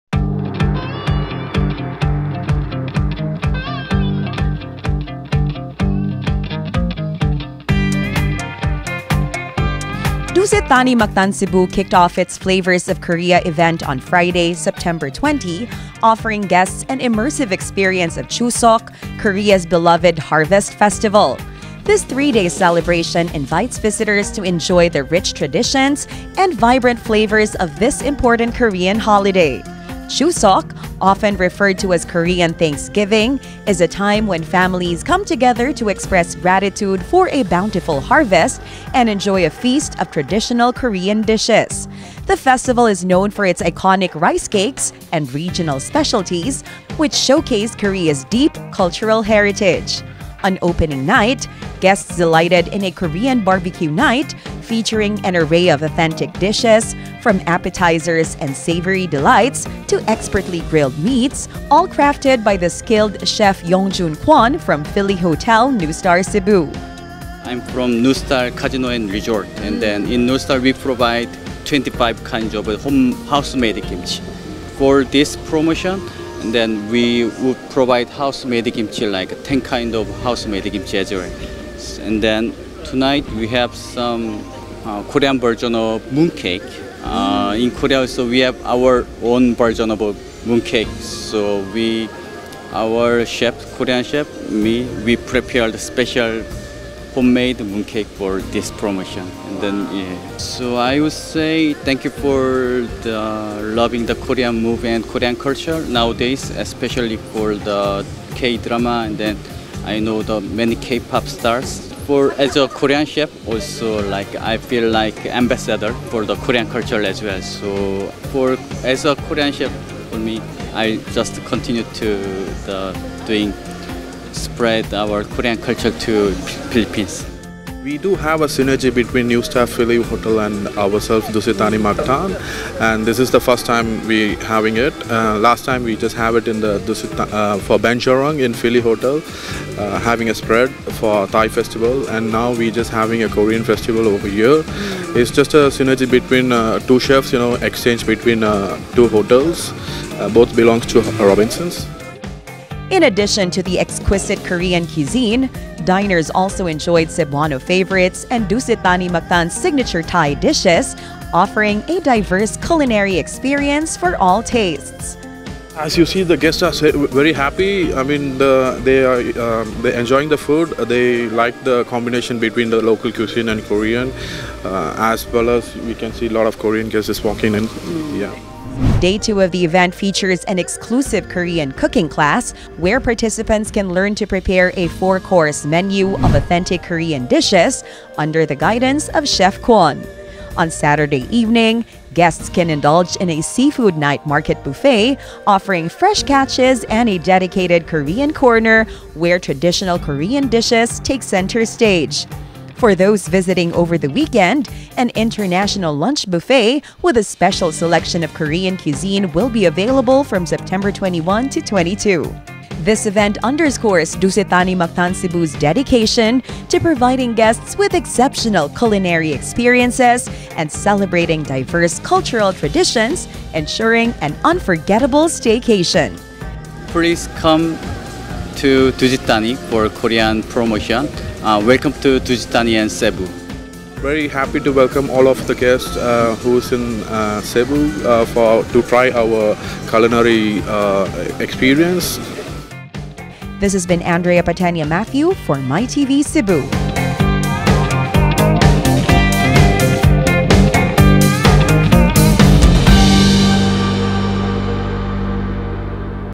𝗠𝘆 𝗛𝗮𝗽𝗽𝗲𝗻𝗶𝗻𝗴𝘀: 𝗖𝗲𝗹𝗲𝗯𝗿𝗮𝘁𝗲 𝘁𝗵𝗲 𝗙𝗹𝗮𝘃𝗼𝘂𝗿𝘀 𝗼𝗳 𝗞𝗼𝗿𝗲𝗮 𝗮𝘁 𝗗𝘂𝘀𝗶𝘁 𝗧𝗵𝗮𝗻𝗶 𝗠𝗮𝗰𝘁𝗮𝗻 𝗖𝗲𝗯𝘂
Dusit Thani Mactan Cebu launched its “Flavours of Korea” event on September 20, celebrating Chuseok, Korea's Harvest Festival, with a three-day culinary experience. Guests enjoyed a Korean Barbecue Night, authentic dishes from Chef Yongjun Kwon, and an exclusive cooking class on day two. The event highlights Dusit Thani's commitment to diverse cultural traditions, featuring a mix of Korean, Cebuano, and Thai cuisines for an unforgettable experience.